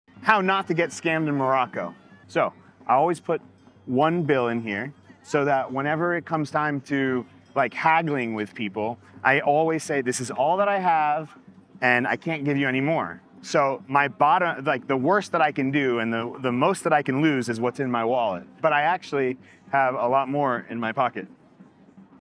#1 tip to avoid getting scammed abroad
Little tip & trick how not get scammed at a market abroad…

Some amount of overpaying is to be accepted but I’m talking about like getting really ripped off.